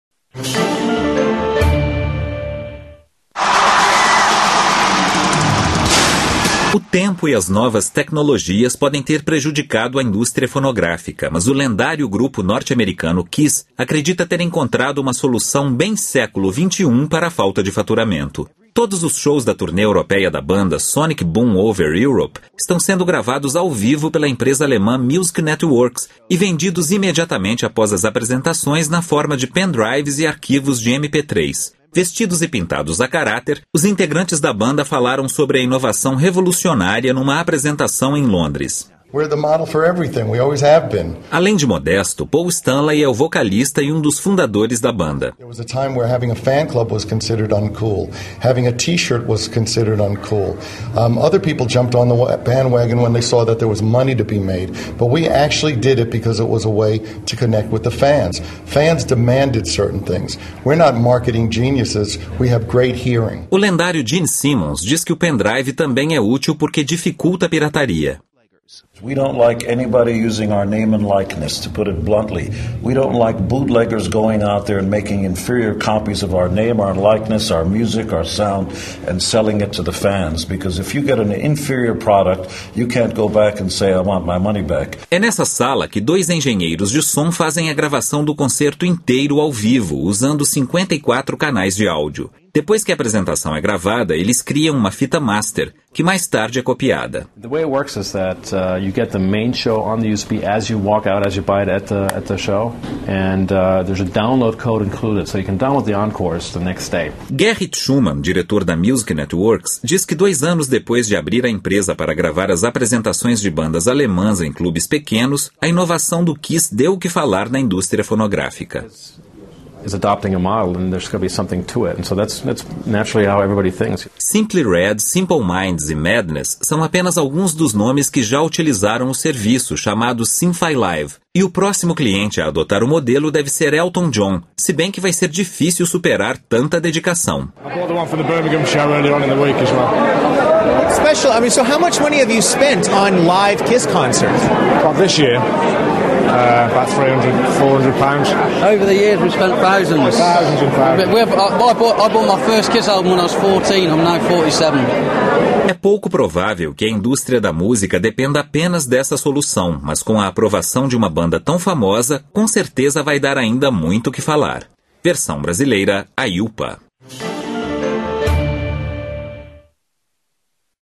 A revolução do KISS
O KISS inova e põe à venda todos os shows de sua turnê europeia em pen drives depois de cada apresentação.